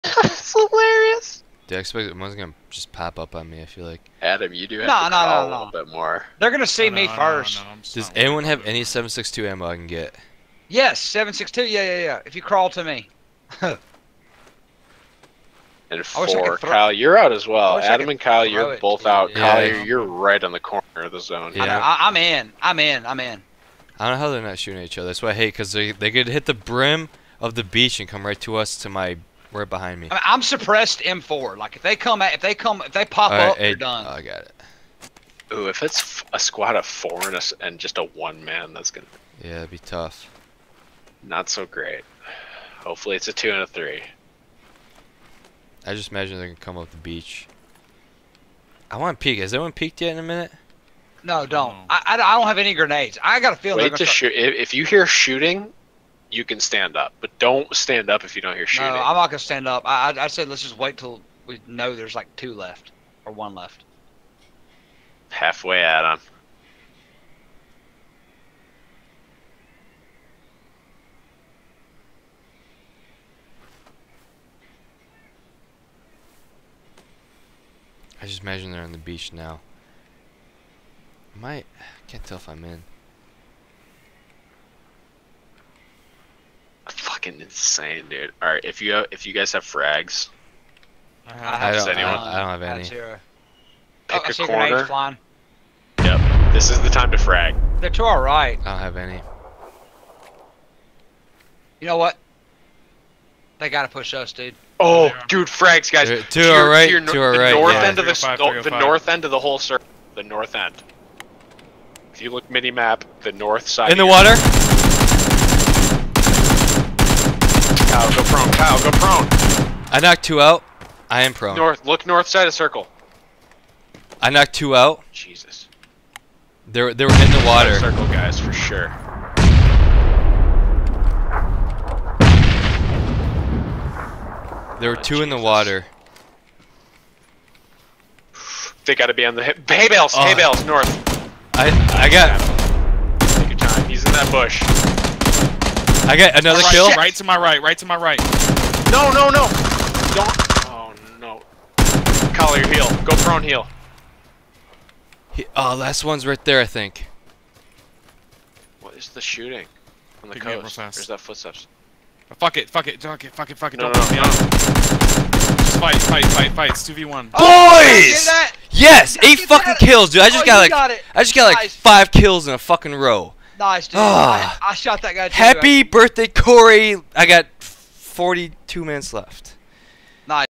Winner Winner TooDrunktoAim, CoreysaurusRex, Colliebama, Adam(pork)
One of the more satisfying winners lately. Like WW1 trench warfare. We waited at this position almost 2 minutes before action took place.

Thanks Collie for the ammo! 20 ammo before hand, you gave me 49 for 69 (lol) total. 14 ammo left. Shot 55 times.